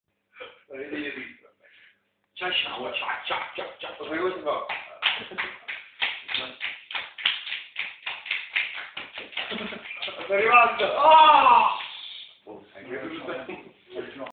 Ciaciare
marco e simone mentre si ciaceano sul letto della nostra stanza al CARIOCA...